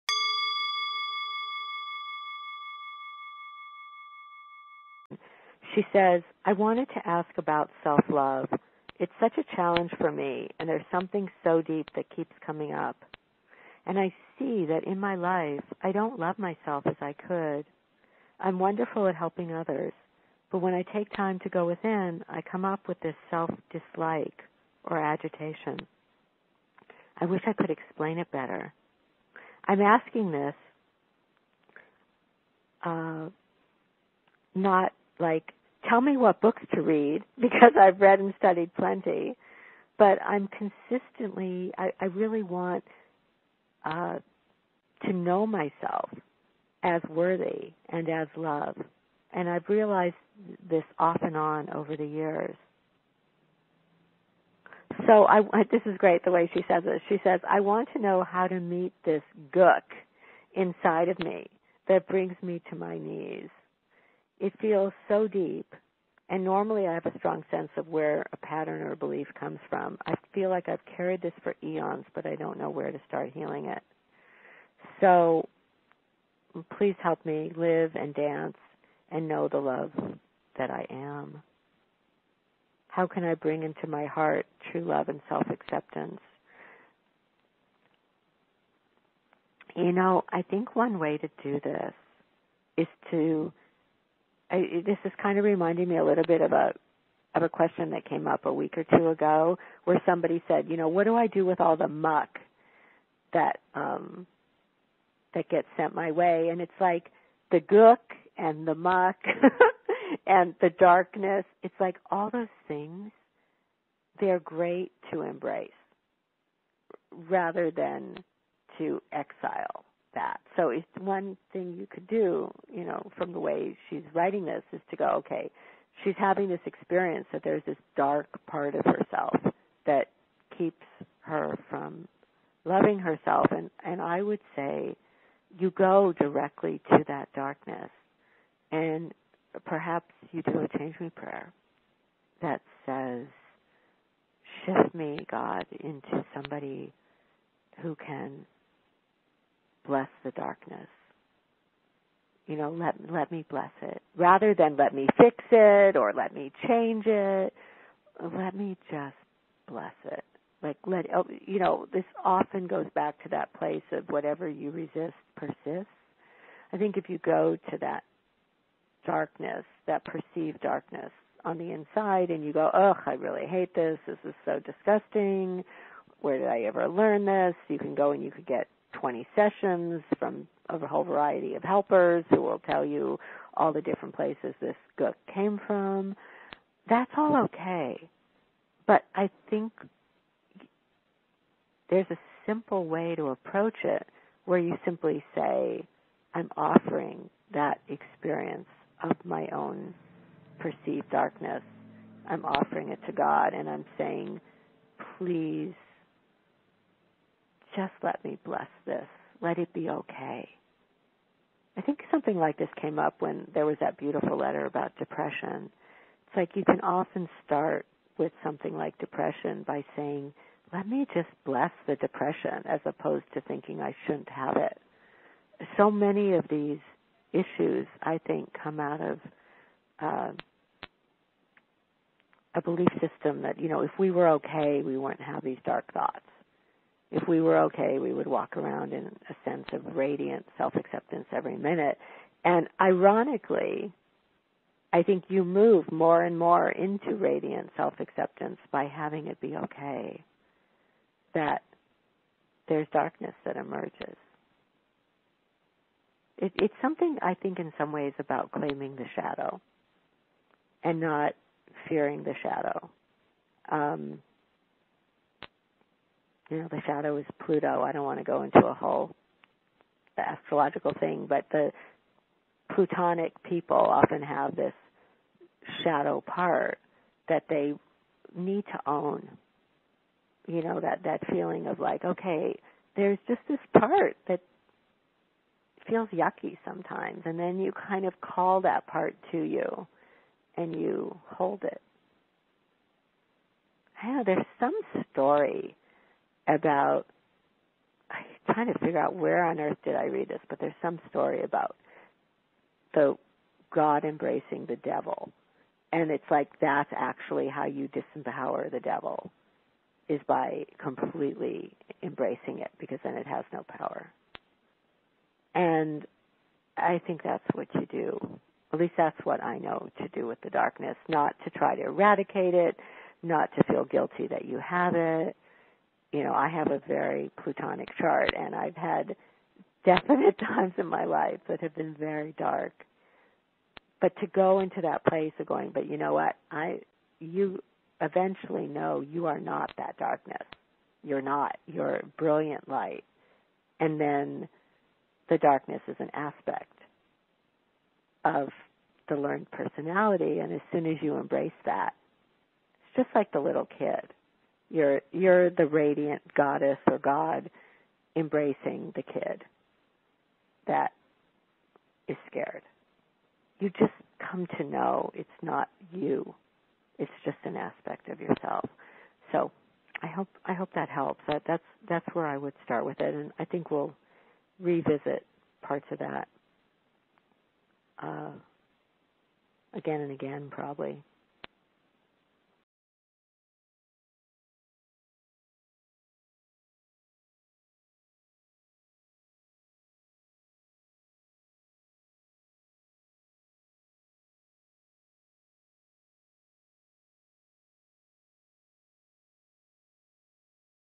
What You Resist, Persists
It is human to shun the dark or undesirable we encounter within us.  But as the old adage goes the more we resist the shadow, the greater power it wields over us. How do we turn our gaze kinder when we look at the undesirable within us? Tosha addresses the question in this excerpt from the 'Living "Outrageous Openness" Forum'.